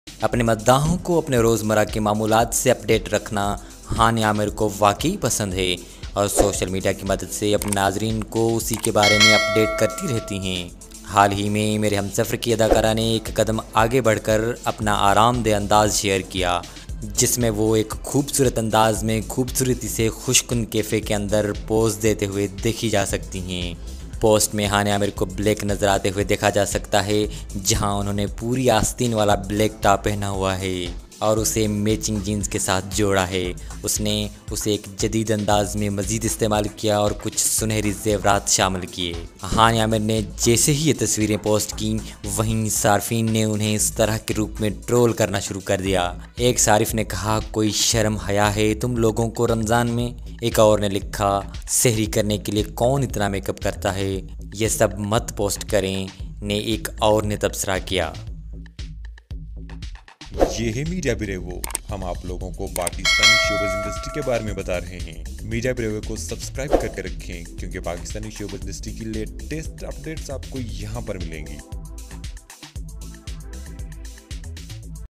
Mere Humsafar Actress Hania Amir Recently Hot Look In Black Top | hania Aamir new dance Video
MEDIA BRAVO

Welcome to @Media Bravo ! Your one stop destination for Everything and Anything around Pakistani Movies , Dramas , Celebrities Television & Movie Stars.